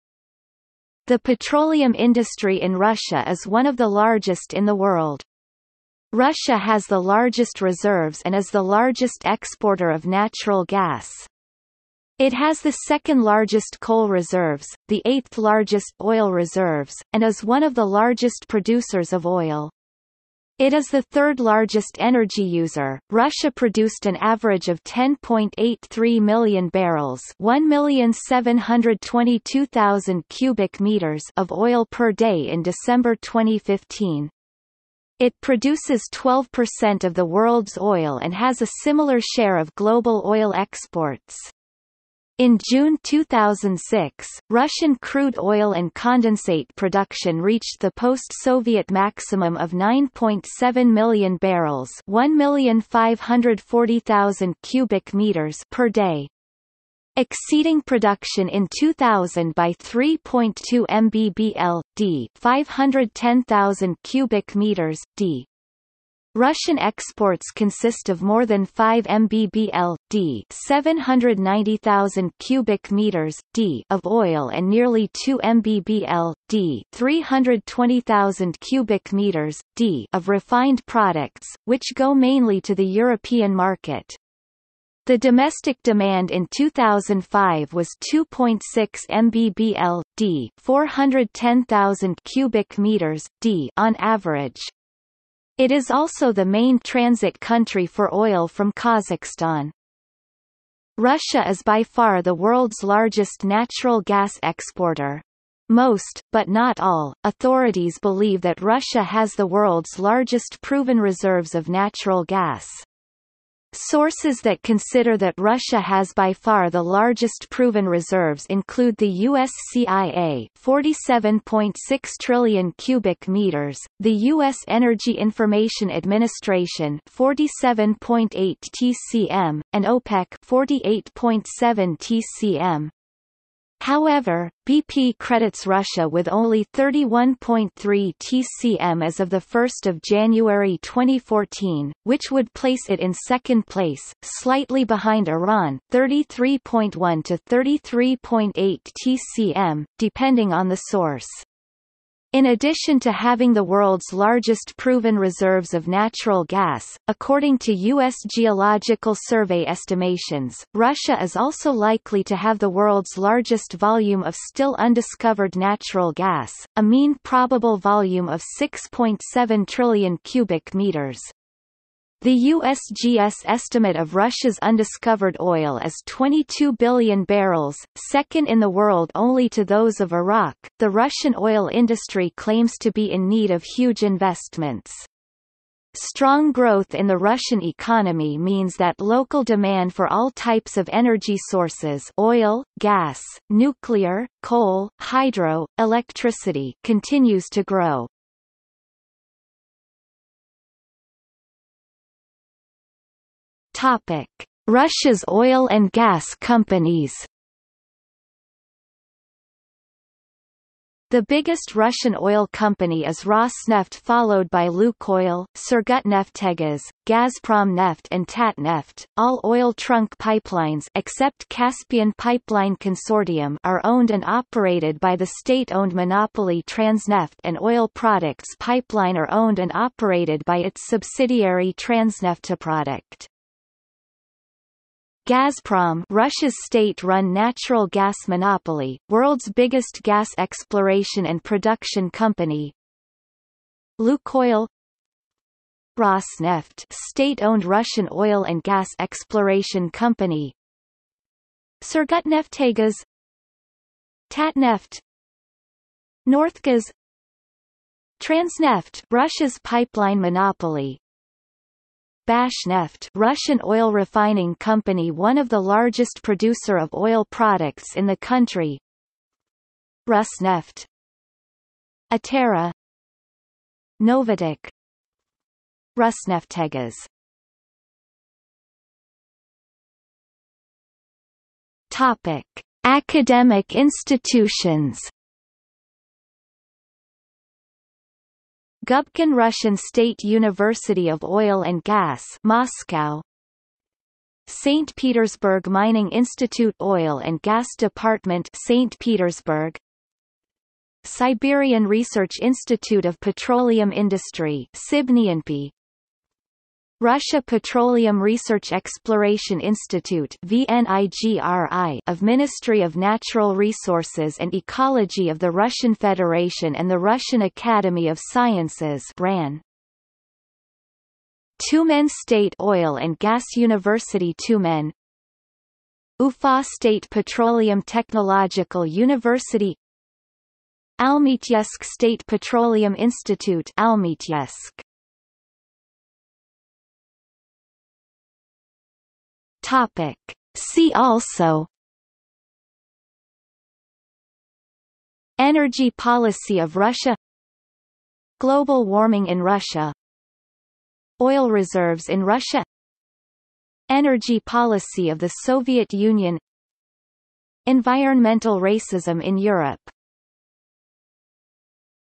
Russian oil industry | Wikipedia audio article
00:03:20 1 Russia's oil and gas companies
00:04:38 2 Academic institutions
00:05:36 3 See also

- Socrates

SUMMARY
The petroleum industry in Russia is one of the largest in the world. Russia has the largest reserves and is the largest exporter of natural gas. It has the second largest coal reserves, the eighth largest oil reserves, and is one of the largest producers of oil. It is the third largest energy user.Russia produced an average of 10.83 million barrels (1,722,000 m3) of oil per day in December 2015. It produces 12% of the world's oil and has a similar share of global oil exports. In June 2006, Russian crude oil and condensate production reached the post-Soviet maximum of 9.7 million barrels (1,540,000 m3) per day. Exceeding production in 2000 by 3.2 Mbbl/d (510,000 m3/d). Russian exports consist of more than 5 Mbbl/d (790,000 m3/d) of oil and nearly 2 Mbbl/d (320,000 m3/d) of refined products, which go mainly to the European market. The domestic demand in 2005 was 2.6 Mbbl/d (410,000 m3/d) on average.  It is also the main transit country for oil from Kazakhstan.
Russia is by far the world's largest natural gas exporter. Most, but not all, authorities believe that Russia has the world's largest proven reserves of natural gas. Sources that consider that Russia has by far the largest proven reserves include the US CIA (47.6 trillion cubic meters), the US Energy Information Administration (47.8 tcm), and OPEC (48.7 tcm). However, BP credits Russia with only 31.3 tcm as of 1 January 2014, which would place it in second place, slightly behind Iran (33.1 to 33.8 tcm, depending on the source). In addition to having the world's largest proven reserves of natural gas, according to US Geological Survey estimations, Russia is also likely to have the world's largest volume of still-undiscovered natural gas: a mean probable volume of 6.7 trillion cubic meters. The USGS estimate of Russia's undiscovered oil is 22 billion barrels, second in the world only to those of Iraq.The Russian oil industry claims to be in need of huge investments. Strong growth in the Russian economy means that local demand for all types of energy sources (oil, gas, nuclear, coal, hydro, electricity) continues to grow.